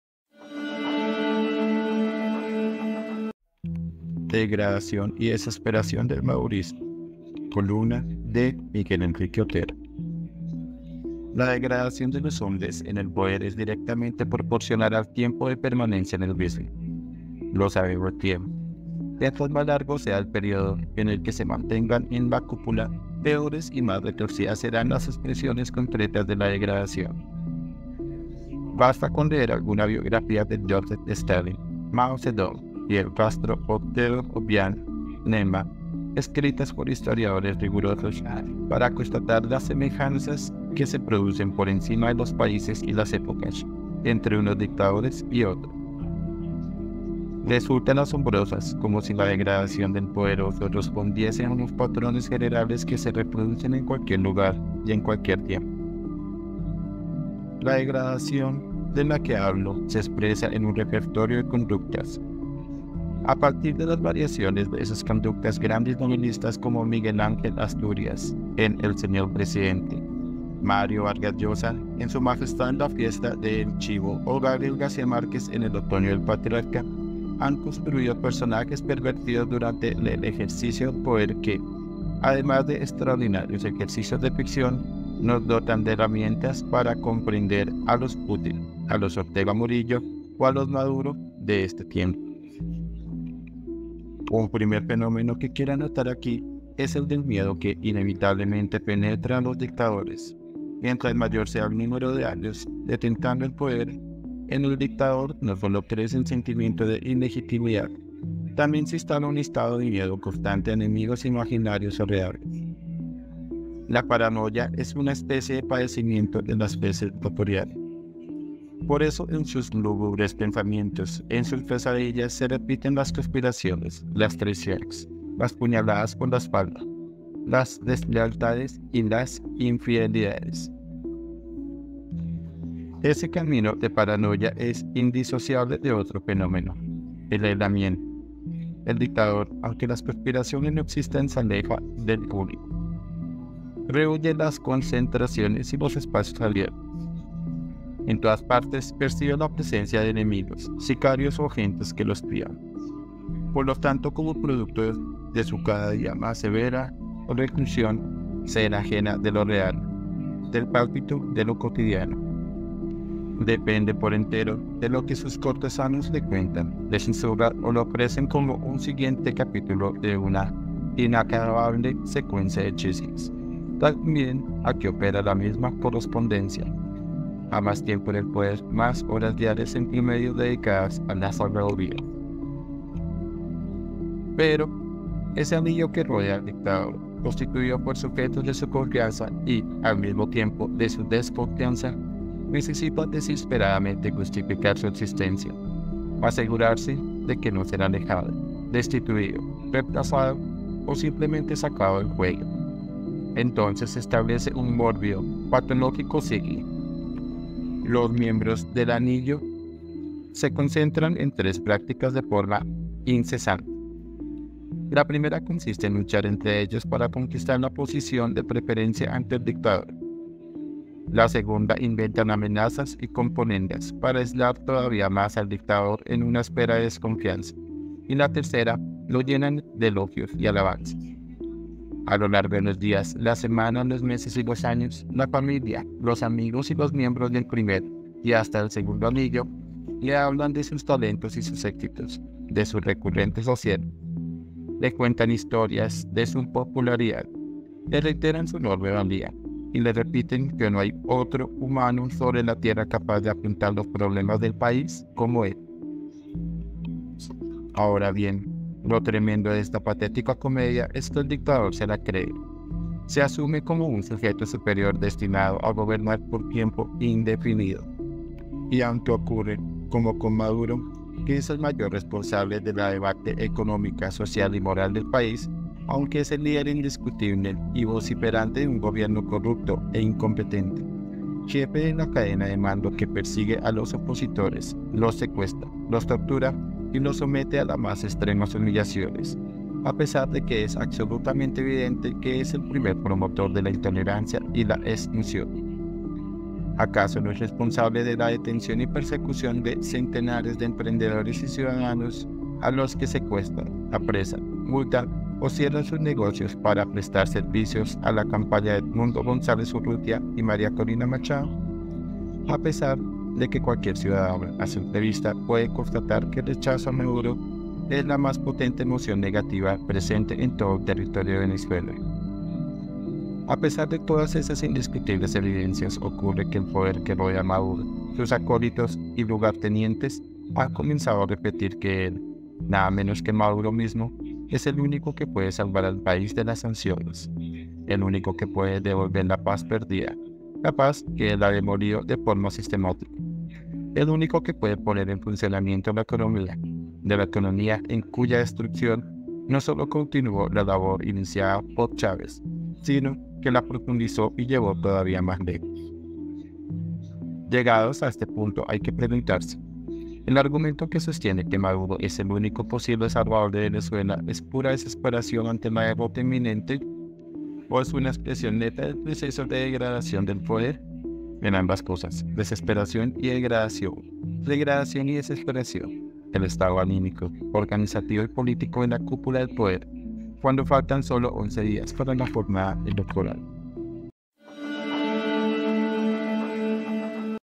DEGRADACIÓN Y DESESPERACIÓN DEL MADURISMO 📢 Columna Miguel Henrique Otero
La degradación de los hombres en el poder es directamente proporcional al tiempo de permanencia en el mismo. Lo sabemos bien: mientras más largo sea el período en que se mantengan en la cúpula, peores y más retorcidas serán las expresiones concretas de la degradación.
columna MIGUEL HENRIQUE OTERO